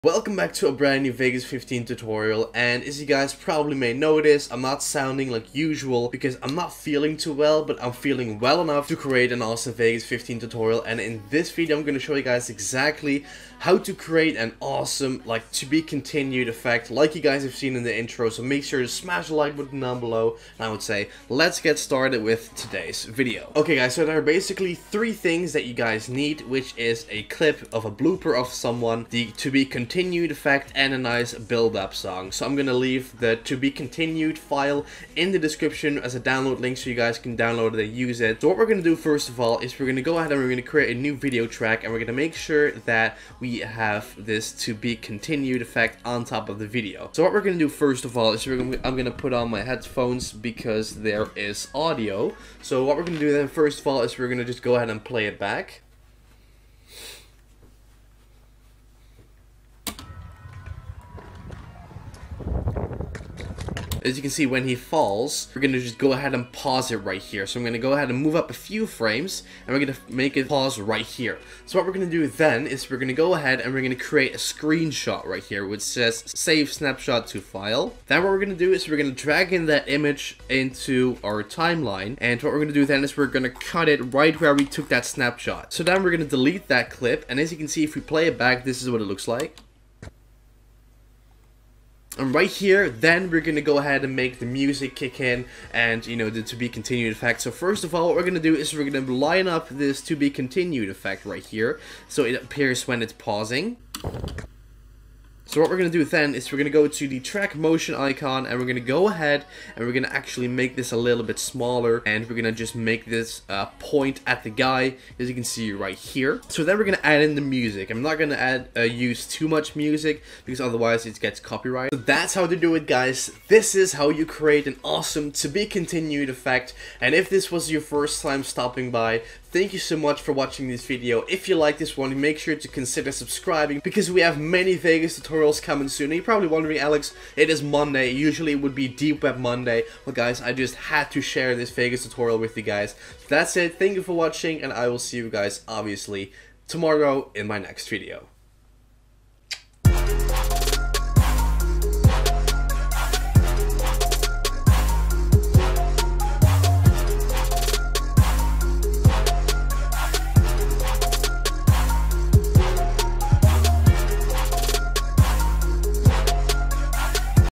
Vegas Pro 15: How To Make A 'To Be Continued' Effect - Tutorial #306
- - Welcome back everyone! Today I'm going to show you how to make a 'to be continued' effect in Vegas Pro 15! Make sure to SMASH that like button below as well! Stick around for more content!